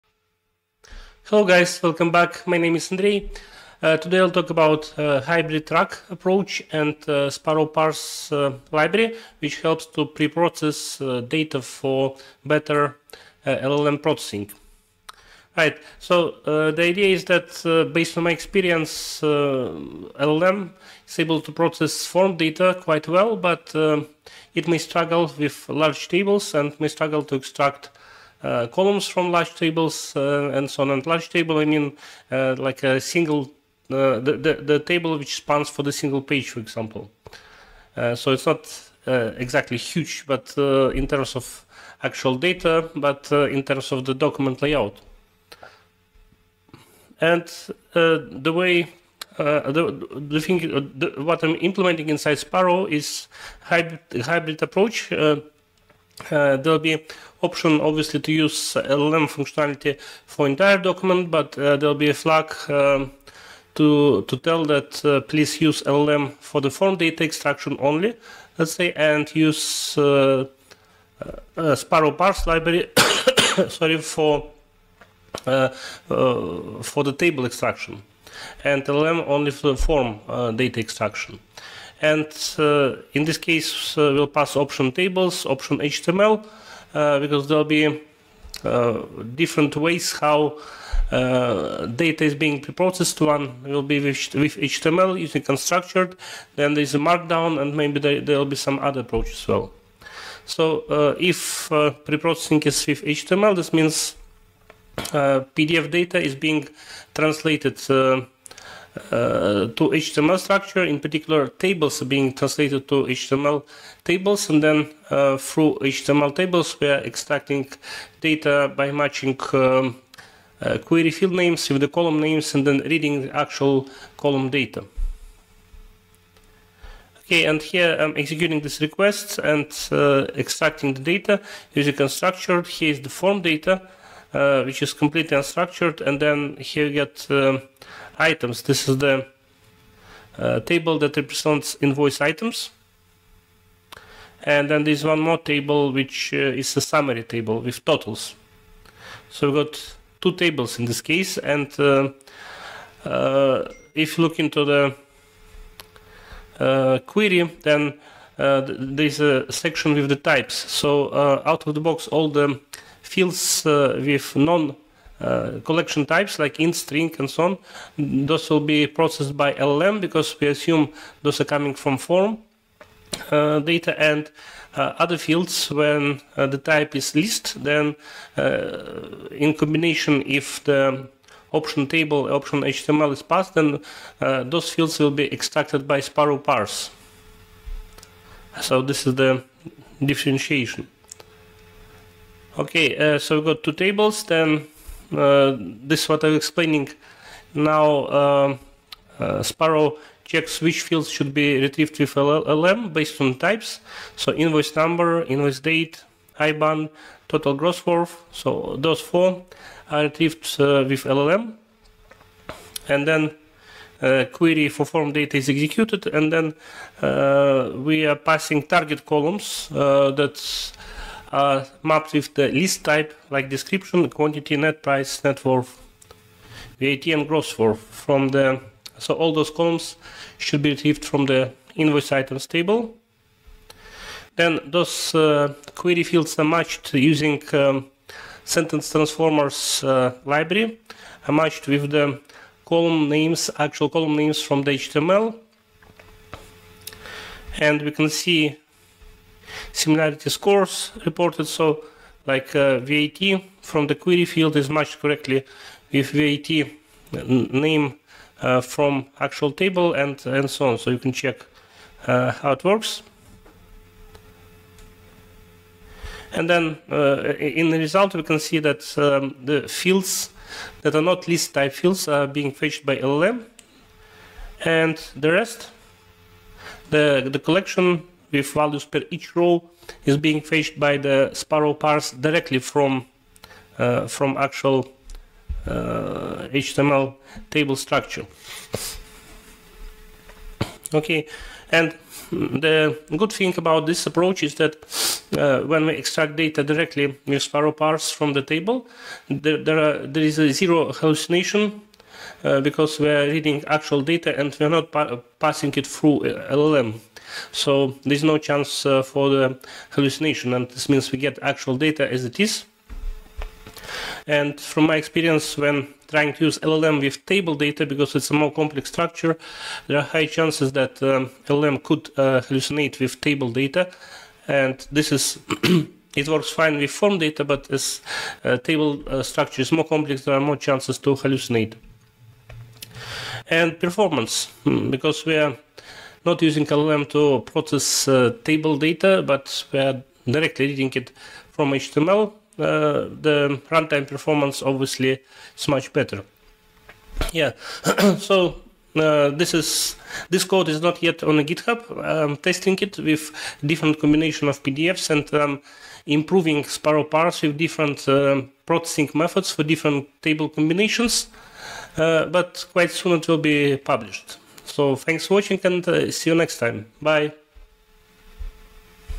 Hybrid RAG with Sparrow Parse
To process complex layout docs and improve data retrieval from invoices or bank statements, we are implementing Sparrow Parse. It works in combination with LLM for form data processing. Table data is converted either into HTML or Markdown formats and extracted directly by Sparrow Parse. I explain Hybrid RAG idea in this video.

0:00 Intro
1:25 Prompt example
2:20 Table processing
3:45 Table column matching
4:33 Response
4:55 Advantages
5:50 Summary